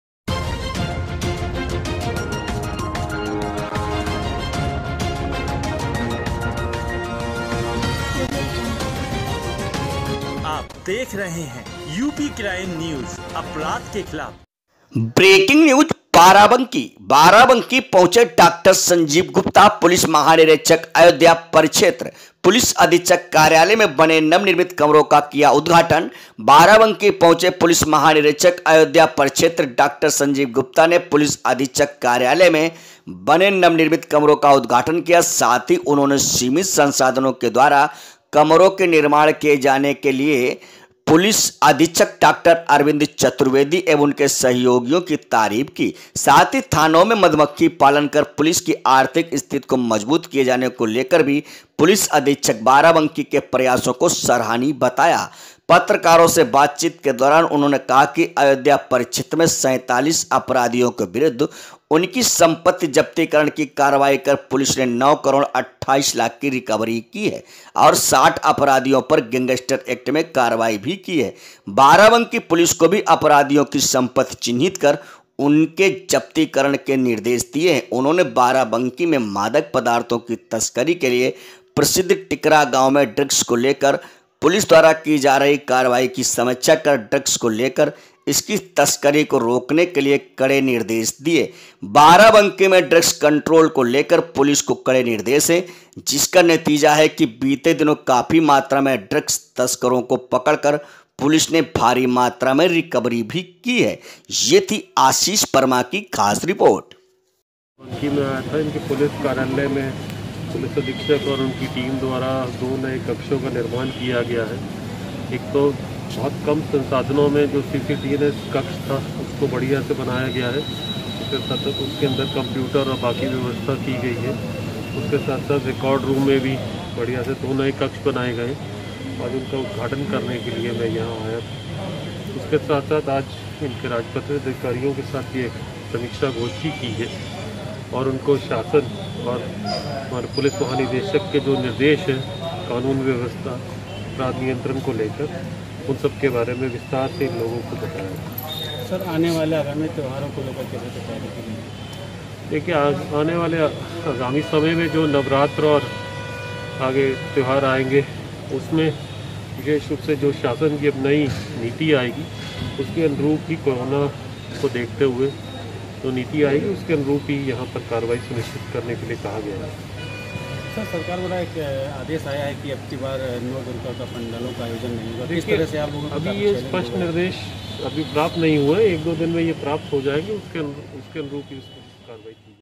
बाराबंकी-बाराबंकी पहुँचे डॉo संजीव गुप्ता (पुलिस महानिरीक्षक) अयोध्या,परिक्षेत्र पुलिस अधीक्षक कार्य
बाराबंकी-बाराबंकी पहुँचे डॉo संजीव गुप्ता (पुलिस महानिरीक्षक) अयोध्या,परिक्षेत्र पुलिस अधीक्षक कार्यालय में बने नव निर्मित कमरों का किया उद्घाटन